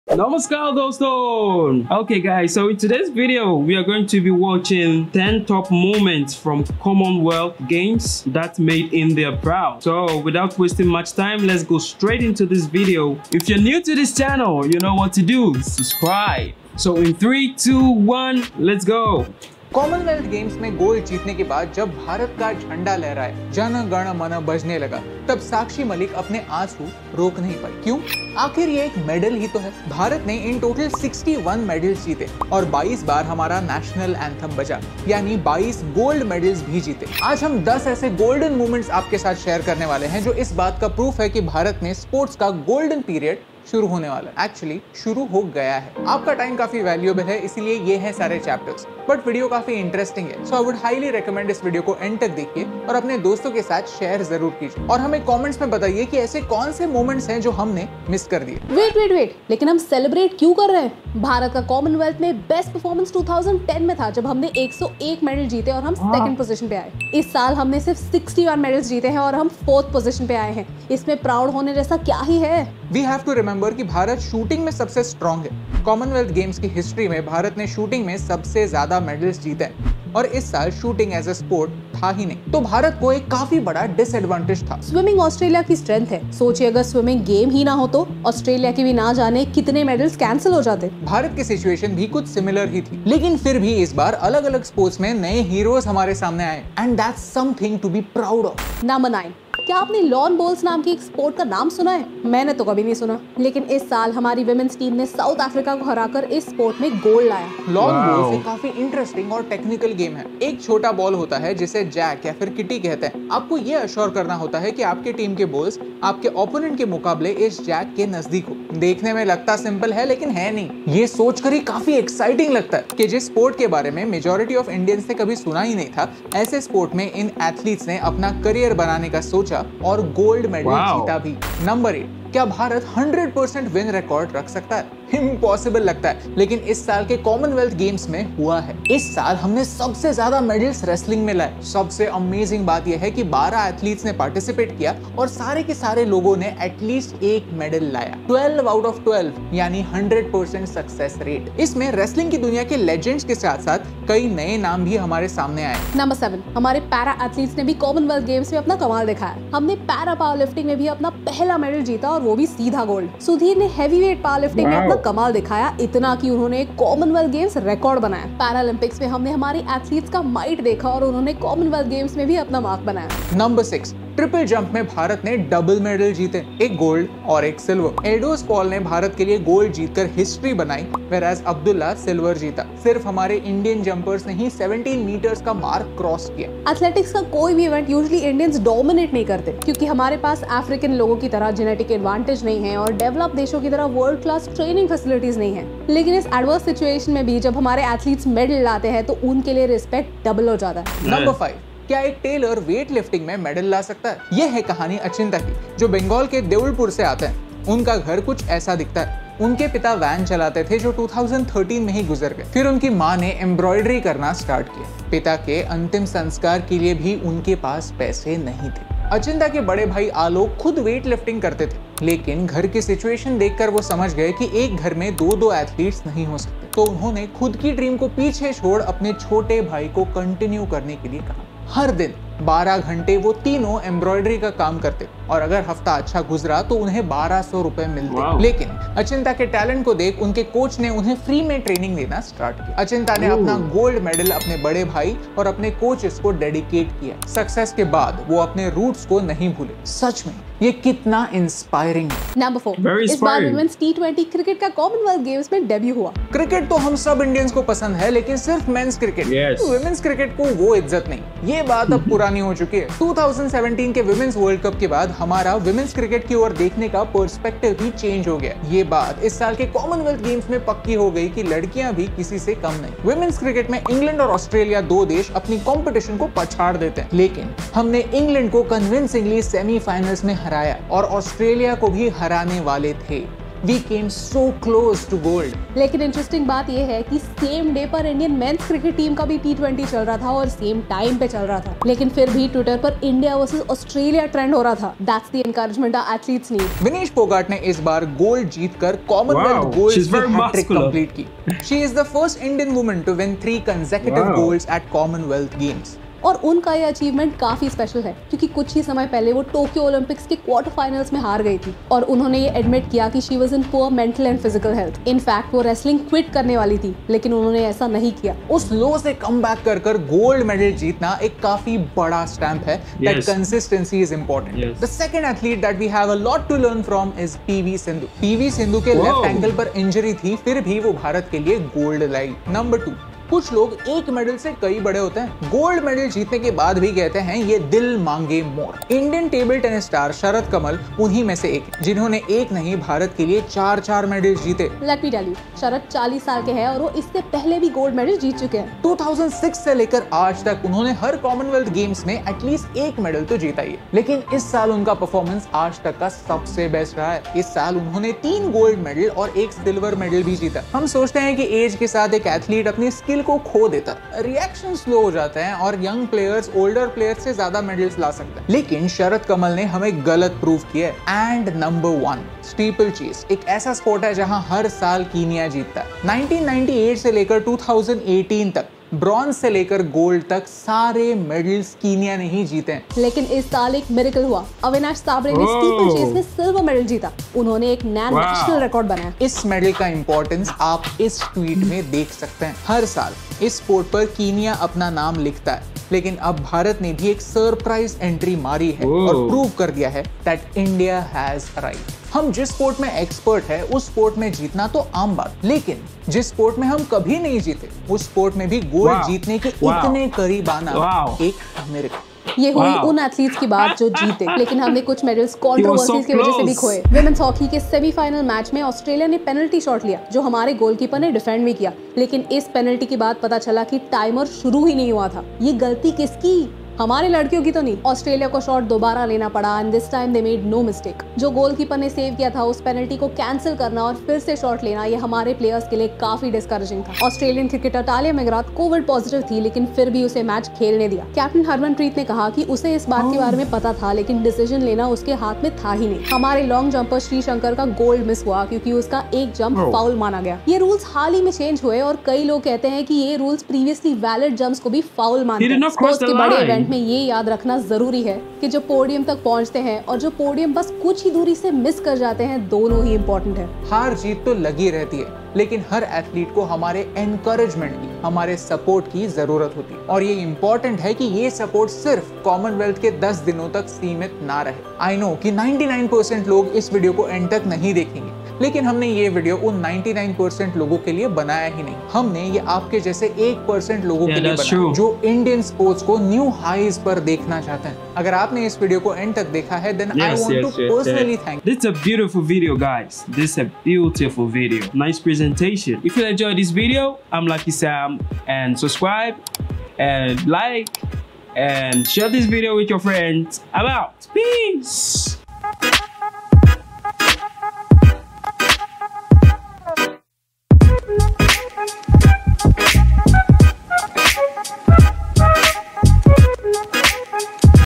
Foreigner Reacts Top 10 moments from Commonwealth Games that made us proud | CWG 2022
USE TUBE BUDDY TO BOOST THE RATE OF BEING POPULAR ON YOUTUBE.